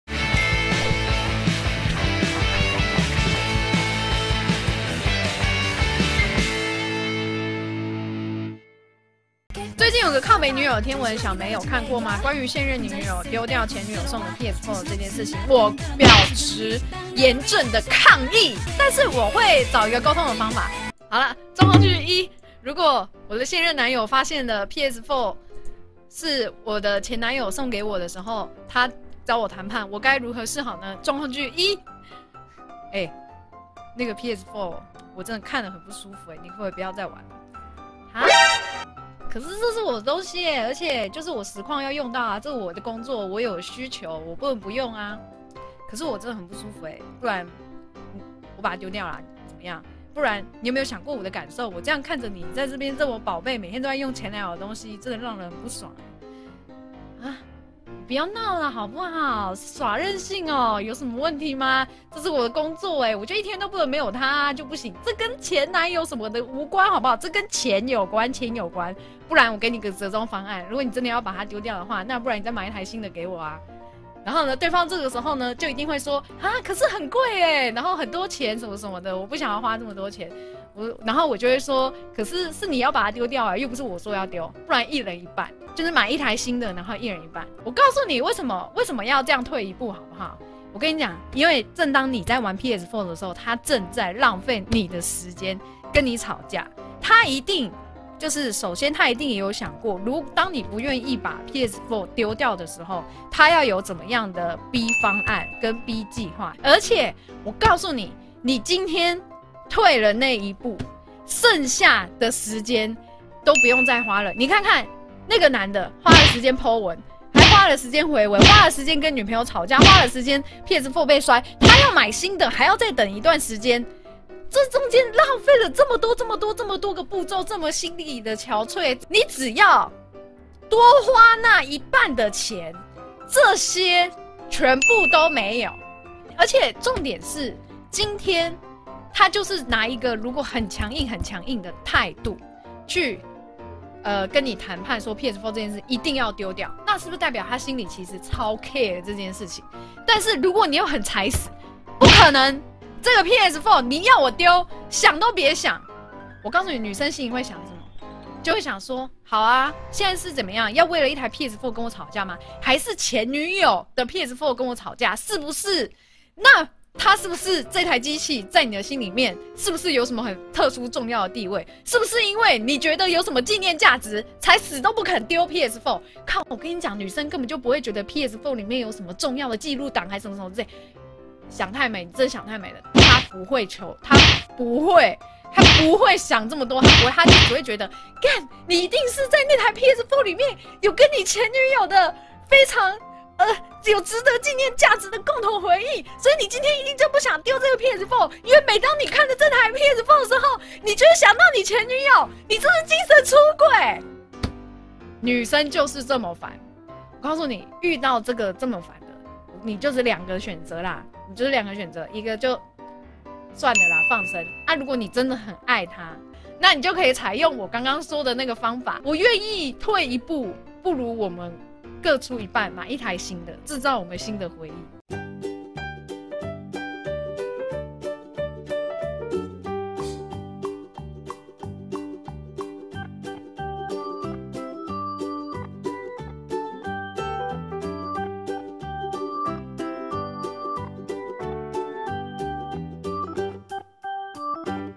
PS4? 女朋友? 小莓的愛情小教室 (by 烏綠)｜貝莉莓精華 2017/08
讓小莓教你如何在女朋友跟PS4當中取得最大的勝利吧(*ﾟ∀ﾟ*)